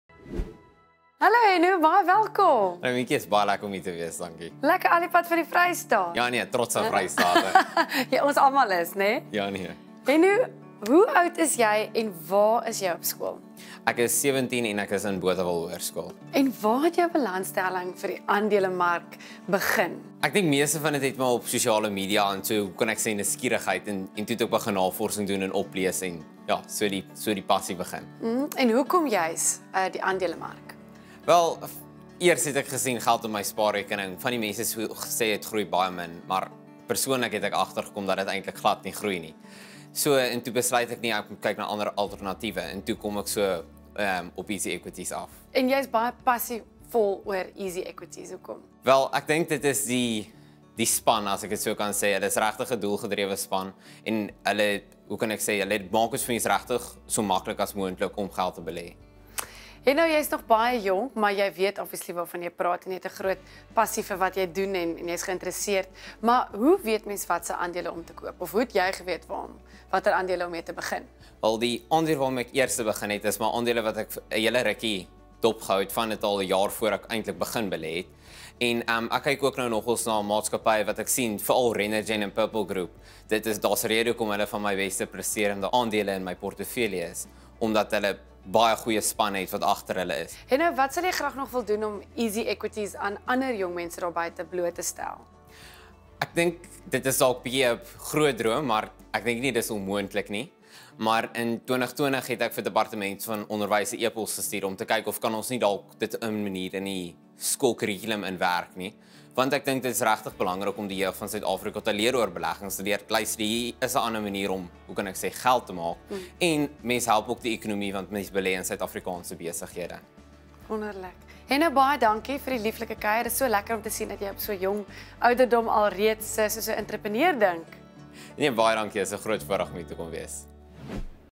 Minki chats to 17 year old EasyVSTR, Henno
Minki chats to Henno about where his passion for investing started and what his dreams are to create awareness around investing amongst the youth of today.

Please note this is filmed in Afrikaans as it aired on an Afrikaans TV show.